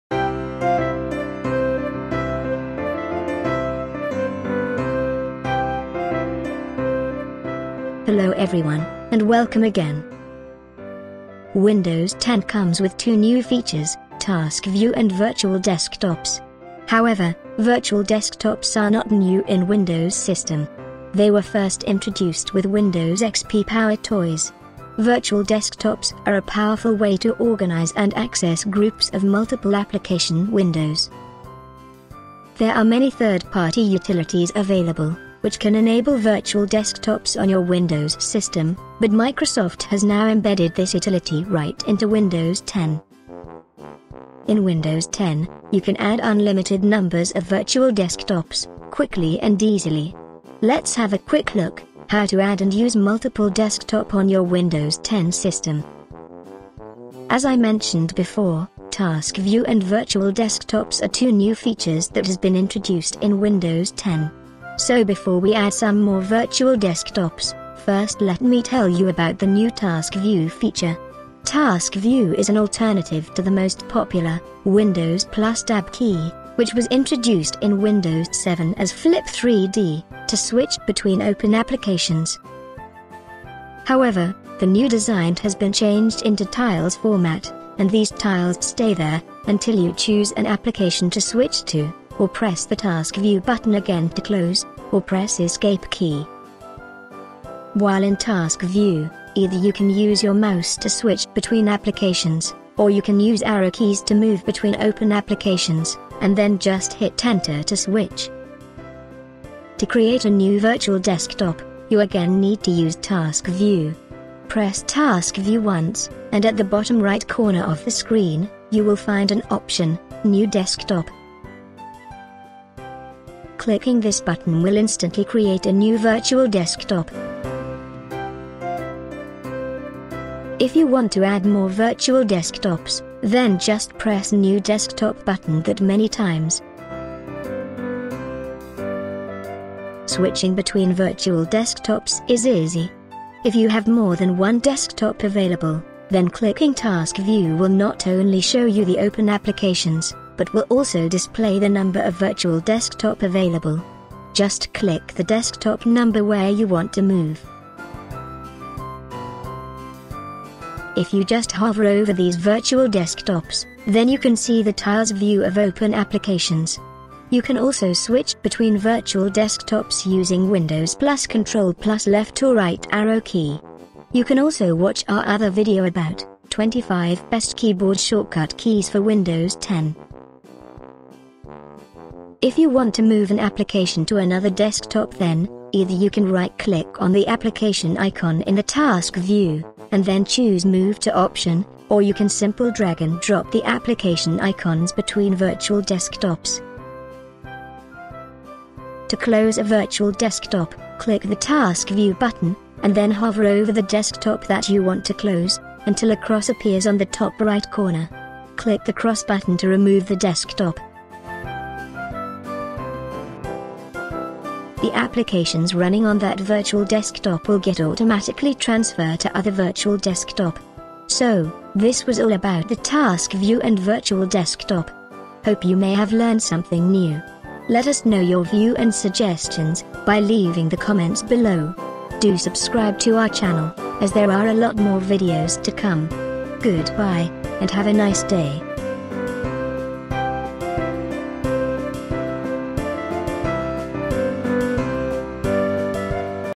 How to Use Task View and Virtual Desktop in Windows 10 Tutorial | The Teacher
Microsoft has introduced two new features in Windows 10: Virtual Desktop and Task View. However, Virtual Desktop was first introduced with Windows XP Power Toys but it now has been embedded into Windows 10. You can organize and group your application on different virtual desktops. Task View is an alternative to the Windows + Tab key to switch between open applications of the current desktop. In this tutorial, you will learn to use Task View and Adding / Switching / Deleting Virtual Desktops. You will also learn to move applications between virtual desktops.

Narrated By:
Emma (TTS)